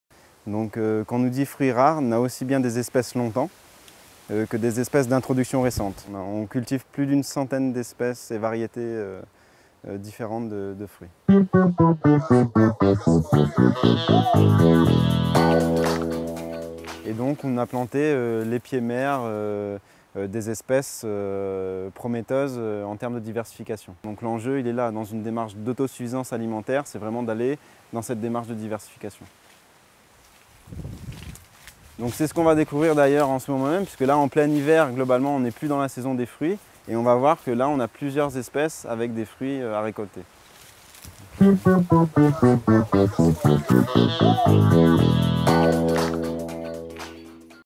Show Devant-Latitude fruitière
Quand récolter le fruit de son travail prend tout son sens...
Avis aux curieux : découvrez ce verger aux saveurs bien particulières
Suivons Samuel, amoureux de son métier à Latitude Fruitière où divers fruits rares y sont cultivés !